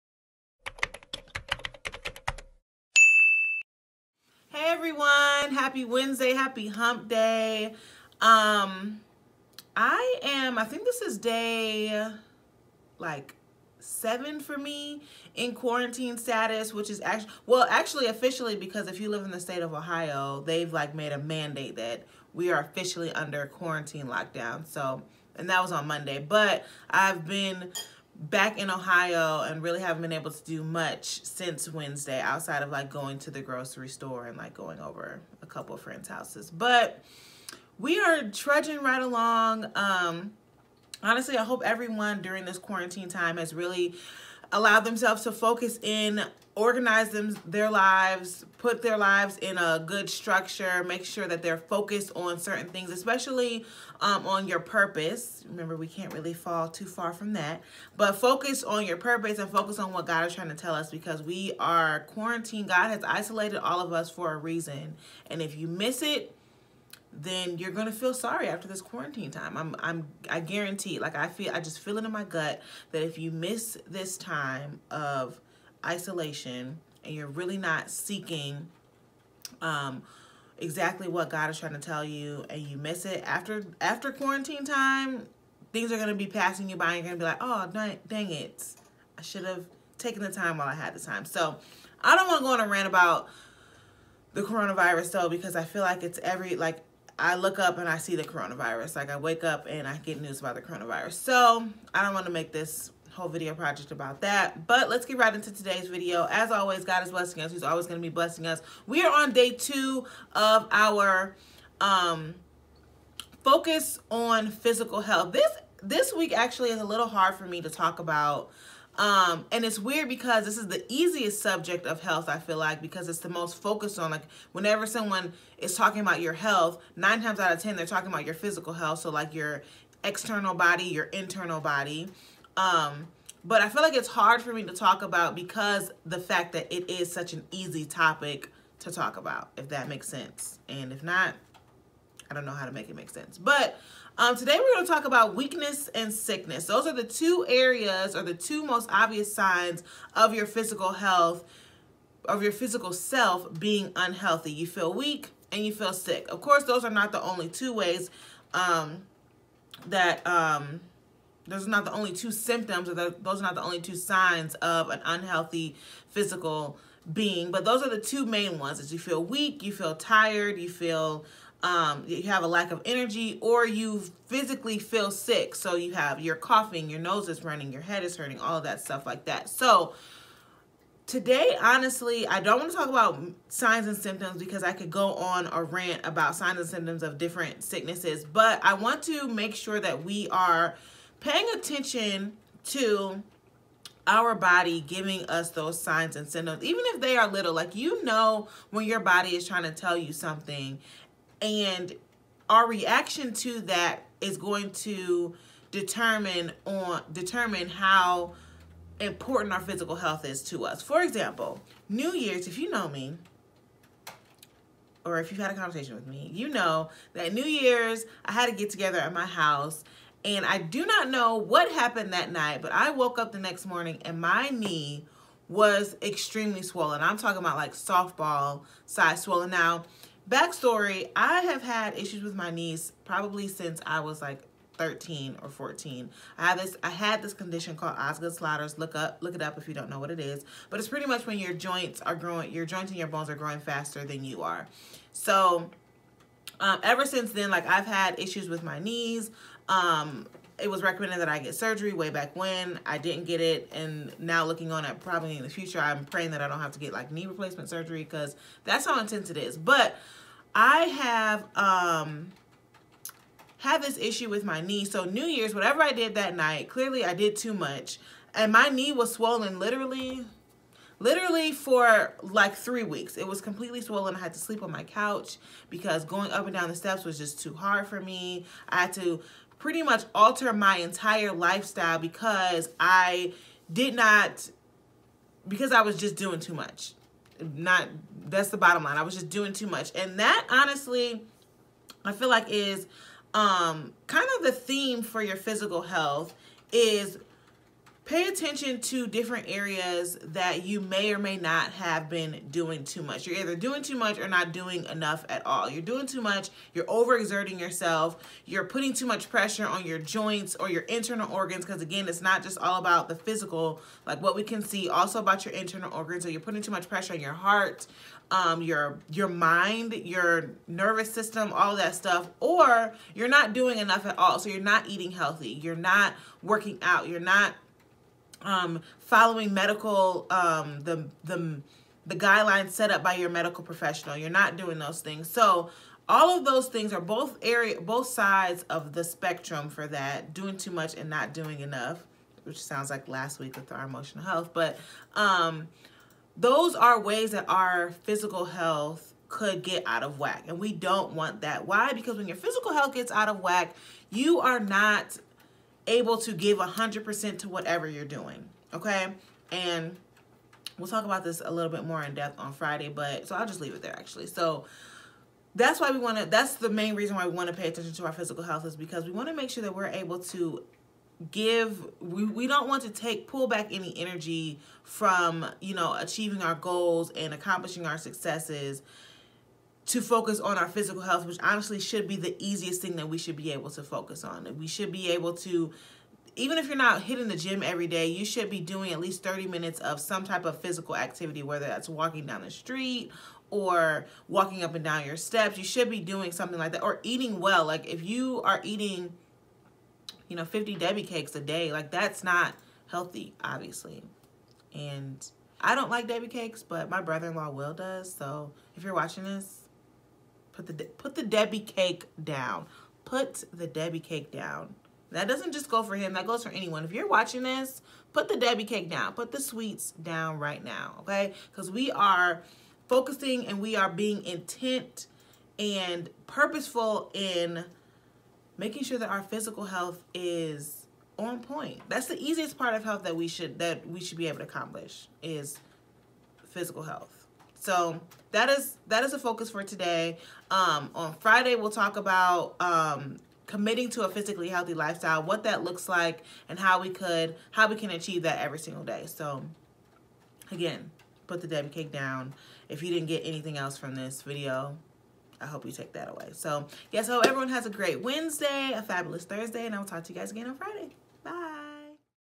Put the Debbie Cakes Down!
Hey everyone! So today’s video title is a bit strange, but the focus of this video was making sure that we are aware of the signs/symptoms of ailing physical health. Also, it’s not just important to be aware of this, but also not ignoring what your body is telling you! And of course, putting the Debbie cakes down ☺️! Make sure you watch today’s video and comment your reactions! Hope you enjoy! Be Blessed!